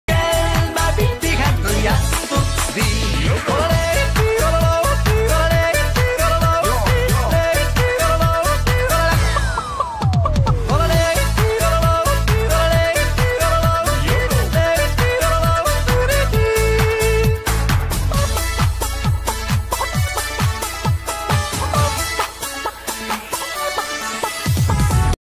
chicken yodelling ||ORIGINAL MEME|| lazy gift
ever since i started my channel my dads always wondered what he’d be like in animation form and I hope this answers his question. I chose this song for a specific reason and i think you know why 😂
love you dad 💕